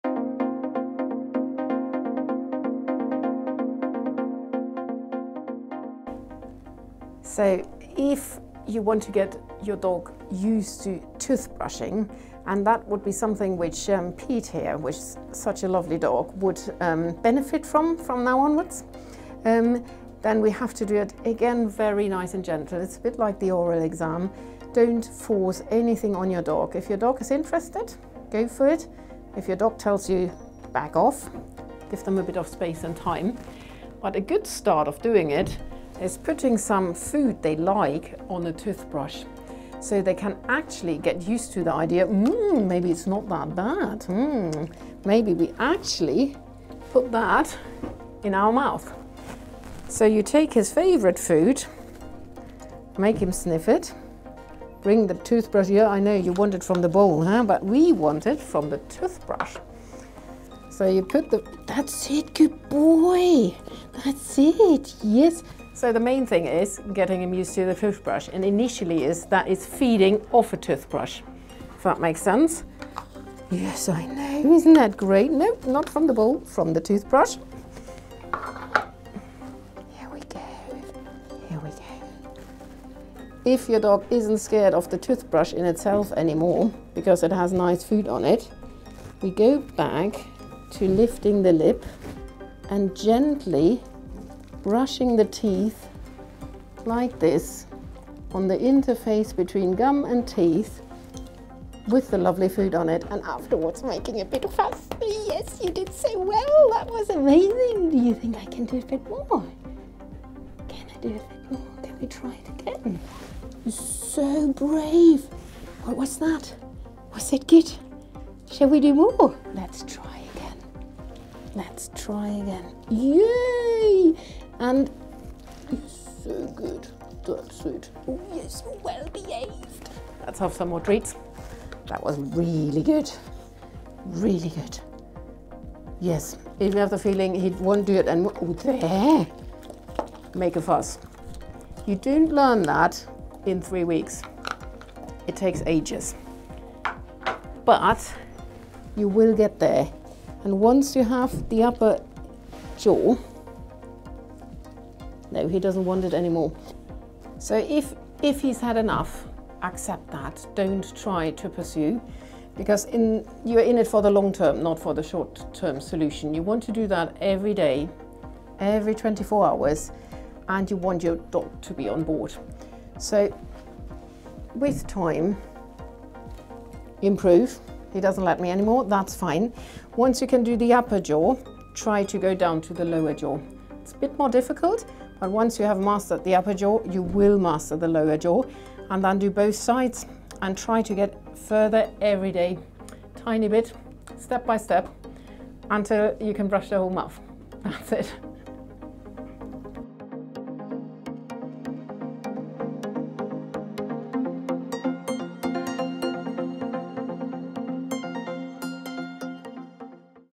A guide to brushing your Greyhound's Teeth - GRI Dental Health Awareness
Just as it is for us humans, it's important to look after your greyhound's teeth and brushing their teeth is one way to help maintain good dental health.

Dr Nora Schwitzer, a Vet & Vet Dentist, shares some really helpful tips on getting your greyhound used to toothbrushing. She shows how you can get your greyhound used to a toothbrush and build their comfort and familiarity with a toothbrush until they are happy for you to use it to brush their teeth.

This video was produced as part of the RCÉ/GRI Dental Health Awareness Month. Learn more on grireland.ie/GreyhoundDentalHealth